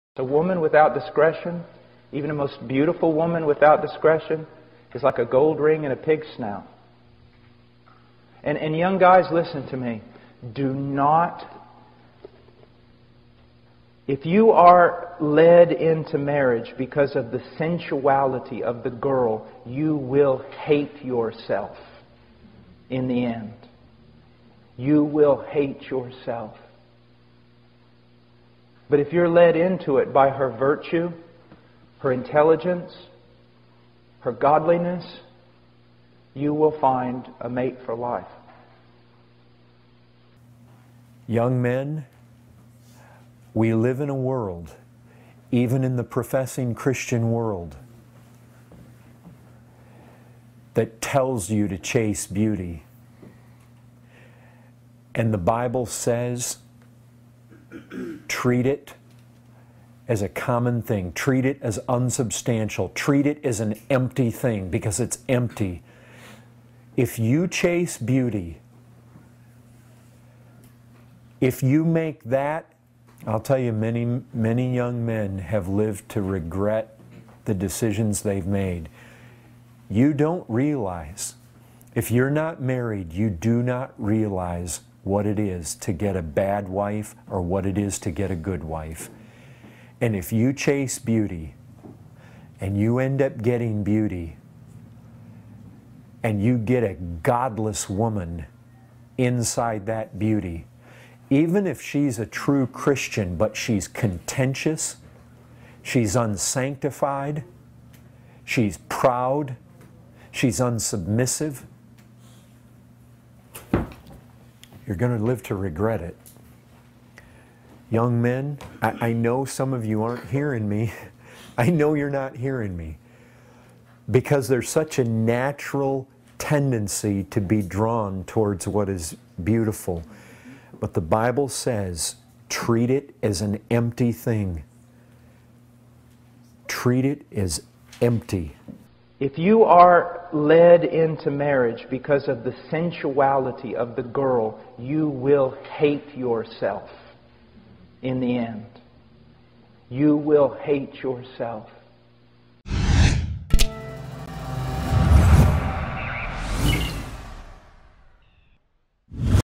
¿Debería casarme con alguien sólo por su belleza externa? - Paul Washer & Tim Conway
Hombres jóvenes, vivimos en un mundo que nos enseña a perseguir la belleza externa de una mujer. La Biblia dice: toma todo eso como algo común, tómalo como algo superficial,como algo vacío, ¡porque es algo vacío!